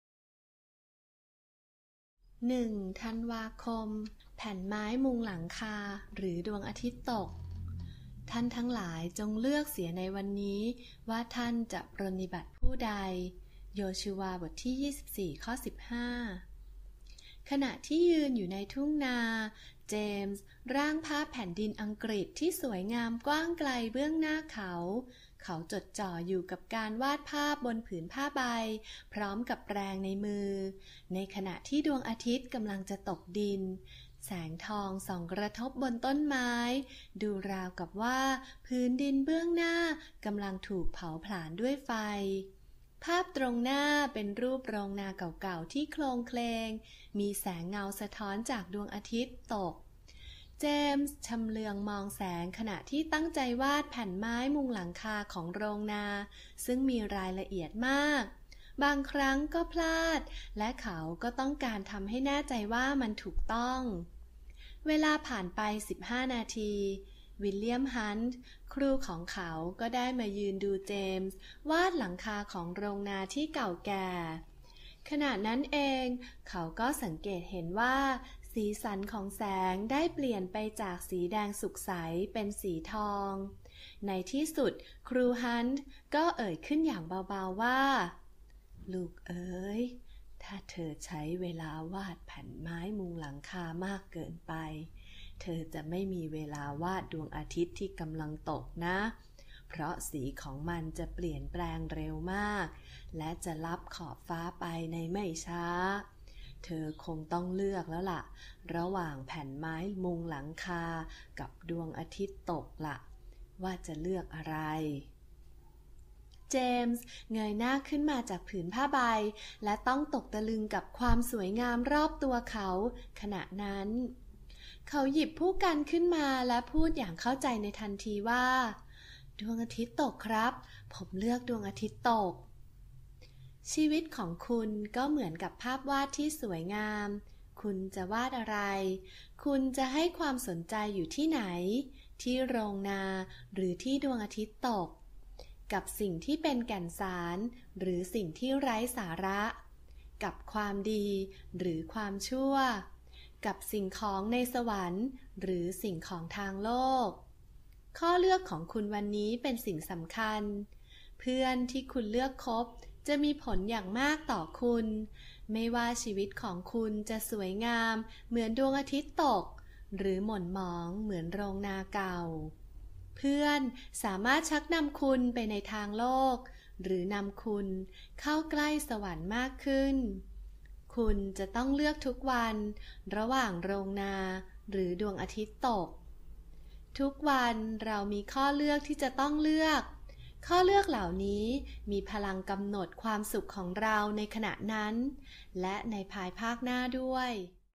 1 ธันวาคม แผ่นไม้มุงหลังคาหรือดวงอาทิตย์ตก
1 ธันวาคม แผ่นไม้มุงหลังคาหรือดวงอาทิตย์ตก
จากหนังสือ เพื่อนเพื่อมิตรภาพ (Friends for Keeps)
โดย โดโรธี อีตัน วัตส์
แปล/เรียบเรียง/อ่านโดย รังษิยา รุ่งตะวันเรืองศรี (หมอรูธ)